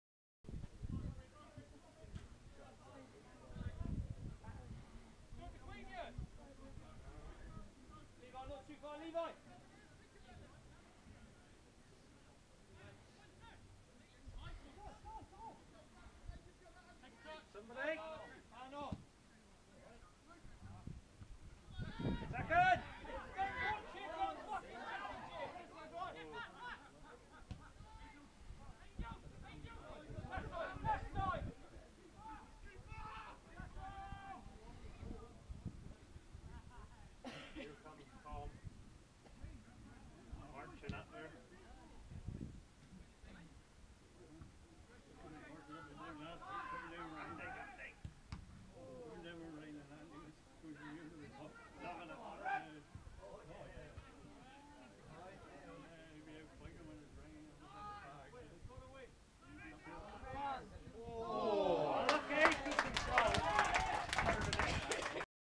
ECPL LEAGUE CUP 2ND ROUND, ST STEPHEN 1st Team 1 v 1 Foxhole Stars 1st Team lose 4-5 on Penalties
Match highlights, Foxhole keeper Andrews makes another save.